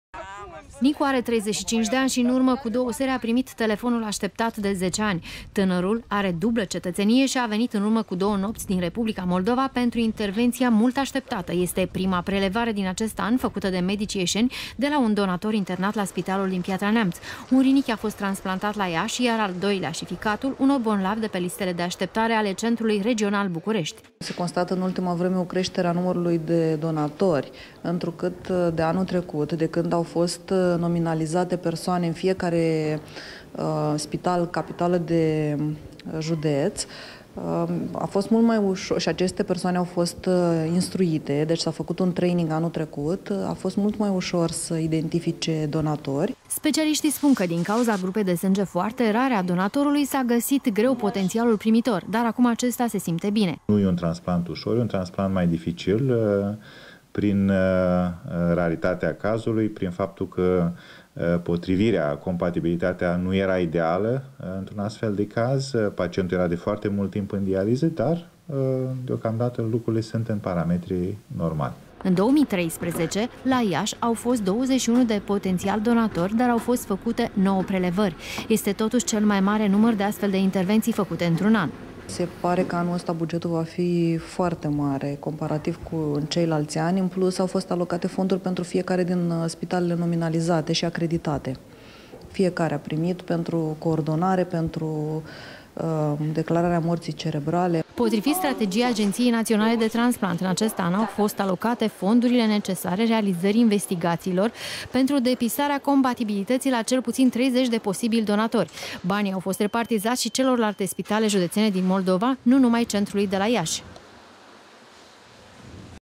Un telefon izbăvitor, după 10 ani de aşteptare
Prima prelevare din acest an făcută de medicii ieşeni constituie o pemieră. Primitorul este un tânăr de 35 de ani din Republica Moldova care face dializă de aproape 10 ani în aşteptarea unui rinichi sănătos. Încă o tentativă nu a putut fi finalizată din cauza stării de sănătate a donatorului. După ce 2013 a fost anul cu cele mai multe prelevări, în acest an se pare că vor fi mai multe intervenţii în condiţiile unei alocări bugetare substanţiale.